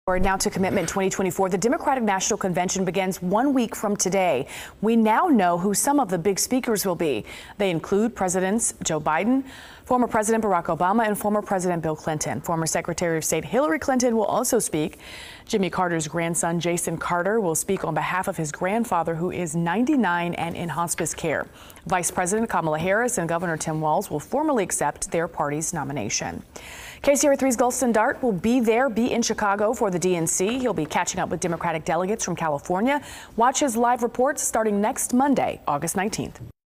Who will speak at the Democratic National Convention?
Here is a look at some of the Democratic National Convention speakers next week.